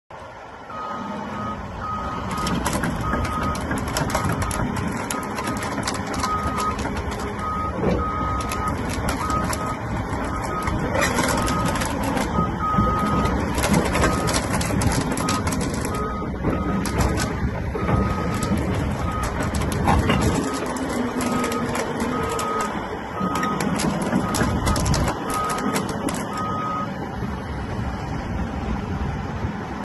2019 CATERPILLAR 349FL For Sale
2019 CAT 349FL AIR CONDITIONER STRAIGHT TRAVEL PEDAL CONTROL PATTERN CHANGER US EPA LABEL ENGINE CAT TIER 4 WITH ONLY 3242 HOURS ULTRA LOW SULFUR DIESEL 11F STICK 35.5 IN  TRIPLE GROUSER TRACK SHOES 75IN DIGGING BUCKET EXTREMELY CLEAN ONLY ONE OWNER EX CATERPILLAR DEALER PRICE $199,999 Dollars